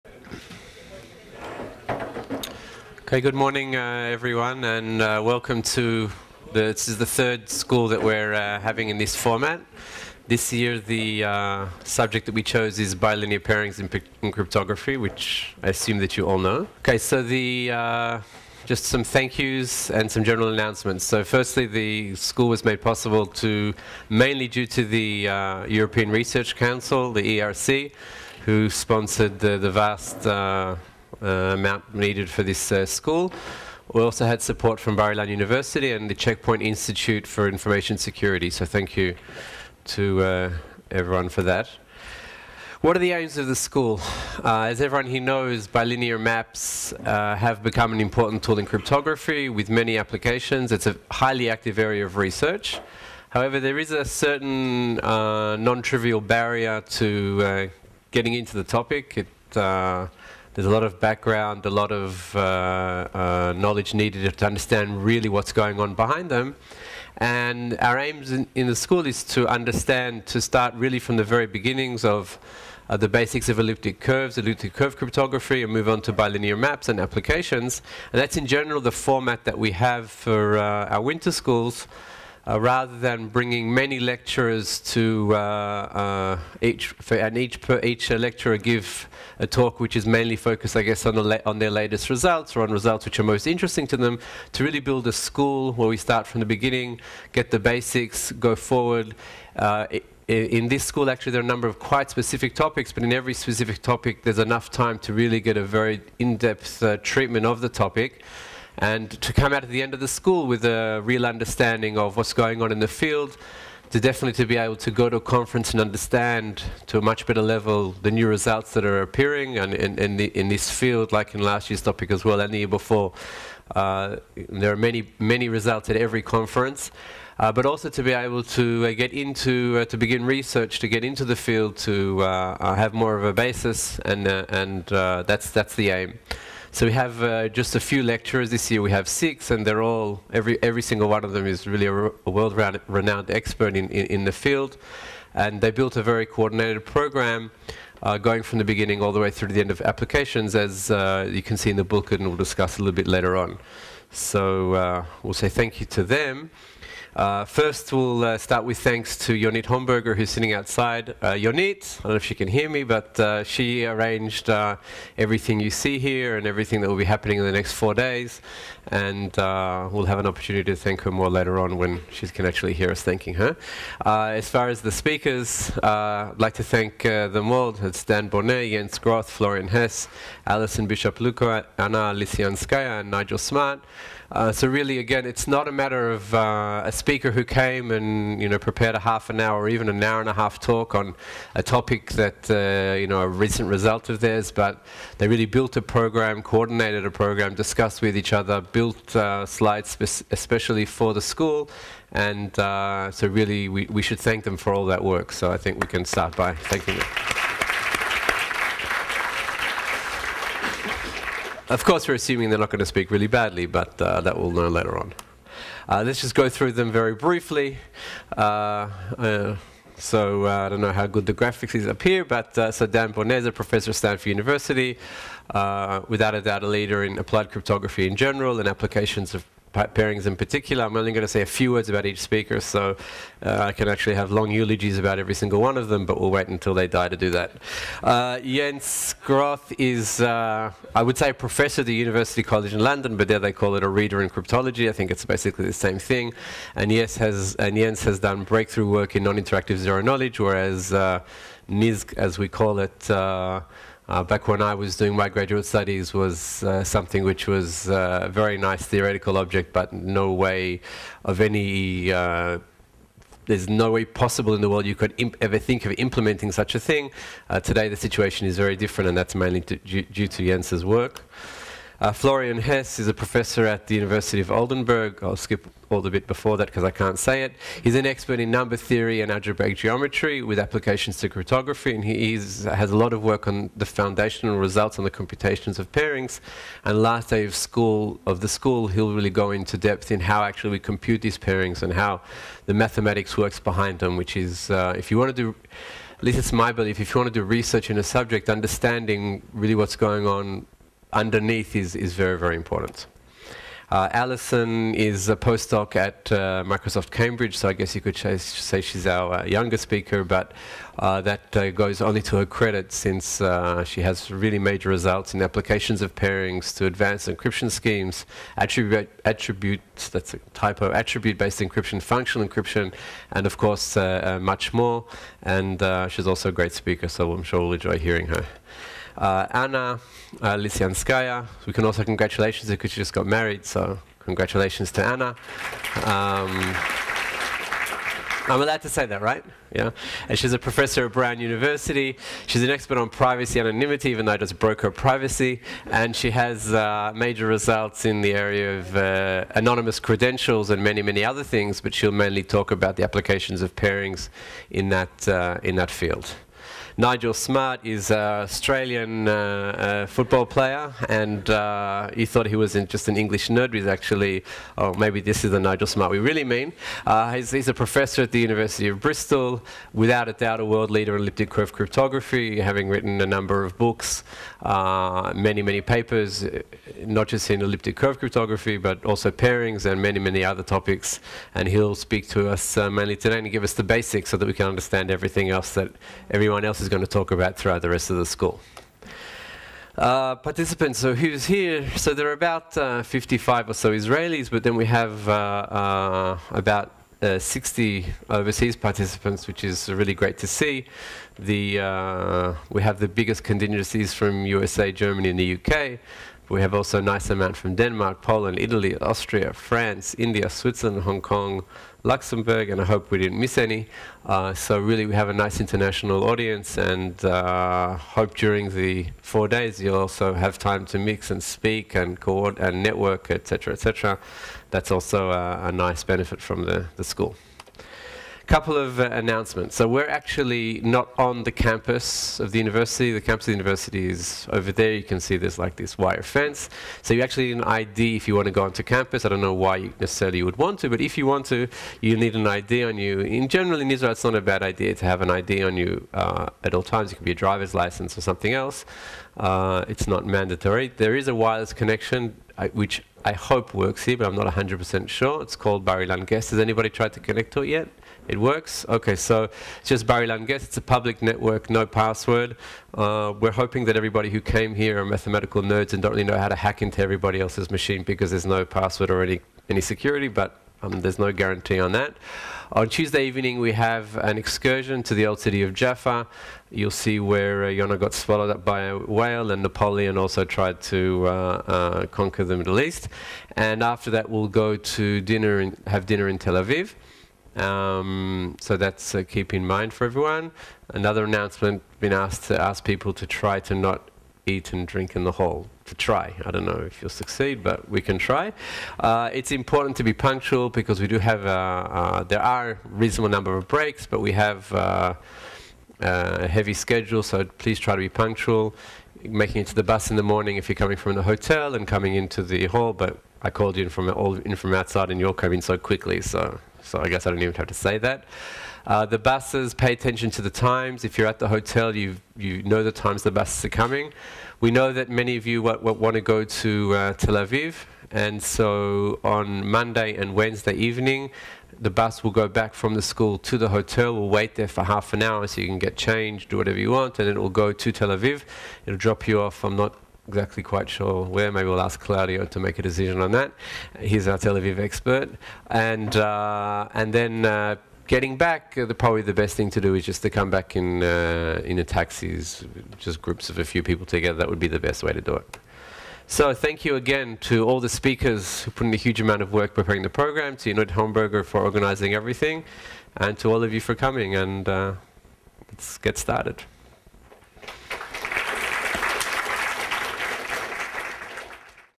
3rd BIU Winter School on Cryptography: Opening Remarks - Yehuda Lindell
The 3rd Bar-Ilan Winter School on Cryptography: Bilinear Pairings in Cryptography, which was held between  February 4th - 7th, 2013.